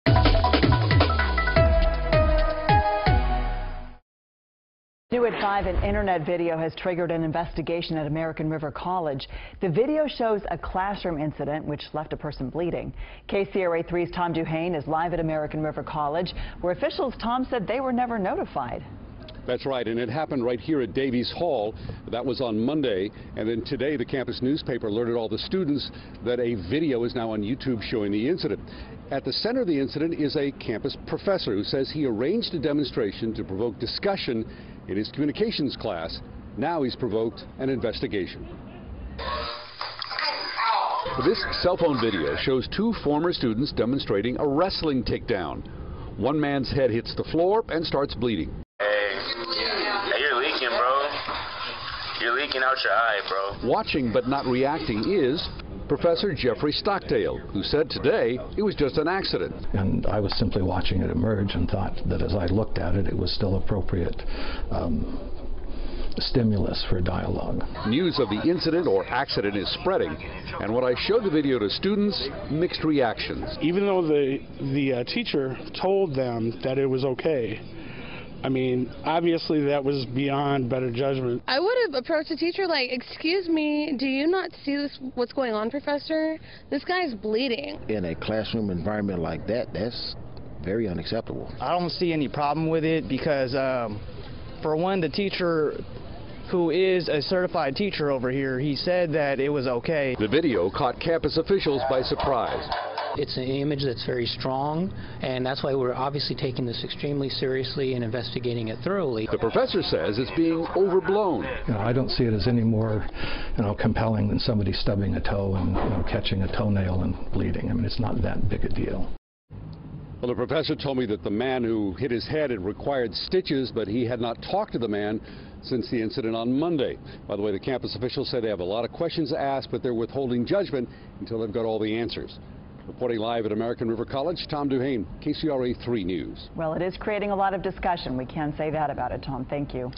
Internet video leads to investigation at American River College
The video shows a classroom incident in which a person is left bleeding.